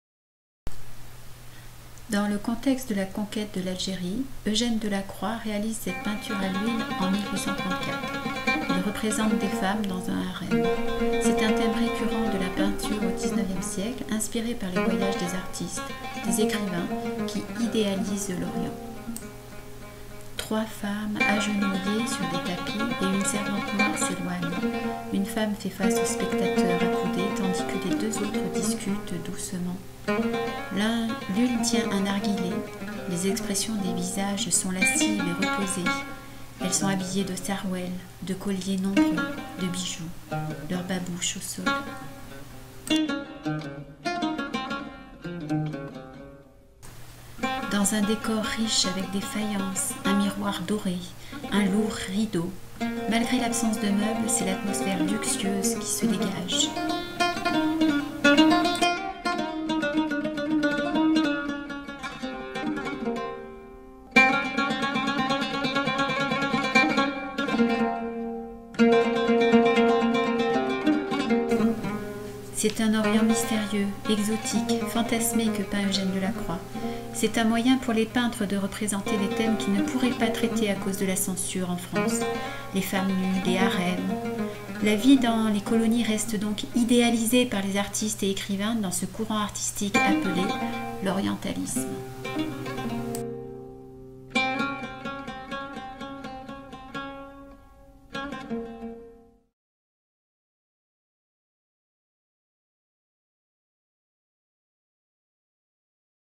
Femmes d'Alger dans leur appartement, Delacroix
Présentation de la peinture "les femmes d'Alger dans leur appartement" d'Eugène Delacroix, en 1834.